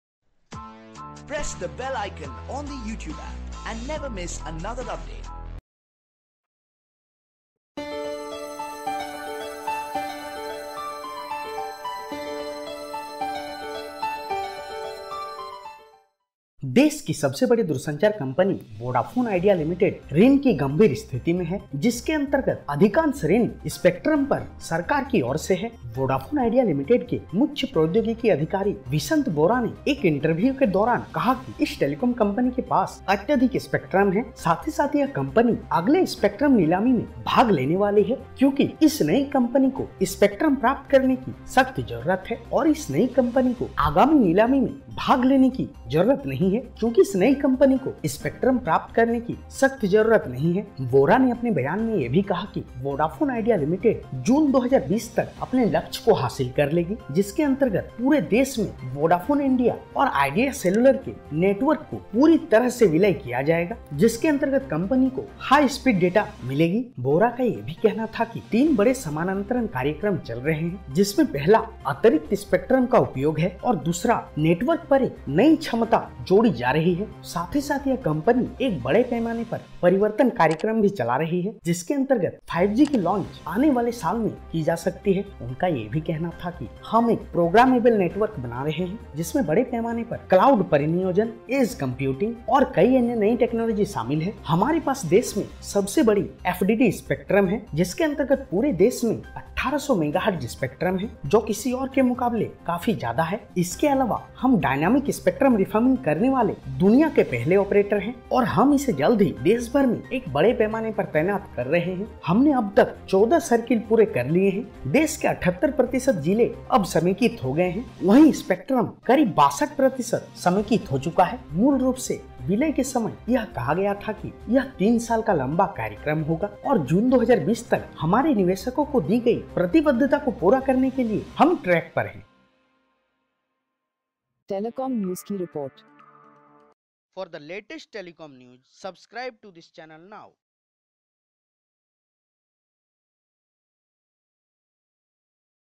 Vodafone Idea Limited To Integrate Whole Network By 2020
Vodafone Idea Limited To Integrate Whole Network By 2020.Vodafone Idea chief technology officer Vishant Vora said that the telco possesses plenty of spectrum and is not in a desperate need to go out and get spectrum in the upcoming auction. He added that the telecom industry is in a serious debt situation and most of the debt is towards the government on spectrum that has been acquired so far.

There are three large parallel programs we are running in our company. One is about de-duplication and releasing synergies which are delivering a lot of benefits both in terms of cost reduction as well as putting extra spectrum to use which has the capacity. Secondly, we are adding a huge new capacity layer on our network. The third thing is we are also running a massive transformation program where we are putting in many of the 5G or next-gen technologies into our 4G plus network.

We are creating a programmable network which will have very large scale cloud deployment, edge computing. We believe this will be the largest deployment in the country. Most likely it will the largest deployment in the world of edge cloud.

Those are three programs running in parallel.

The biggest benefit of the merger was these two companies and their spectrum position. We have the largest amount of FDD spectrum in the country. The biggest benefit is the spectrum benefit.

We have over 1800 MHz of spectrum across the country which is more than anyone else. Out of that total liberalized holding is larger than the other players. We have a long life left on the spectrum, which is also very important. It shows that the long-term roadmap for us is looking better and liabilities going forward are also

Additionally, we were the first operator in the world to do dynamic spectrum refarming. We are deploying this at a larger scale across the country.

We have completed 14 circles so far. 78% of the districts in the country are now consolidated, while 62% of my spectrum has now been consolidated. 70% of my sites have been consolidated and 36% are the top 50% of cities have also been consolidated.

Originally at the time of the merger, we said that it will be a 3-year long program. We are 13 months in, and we are far ahead of our merger plan and we are well in our track to deliver by June 2020, a commitment given to our investors.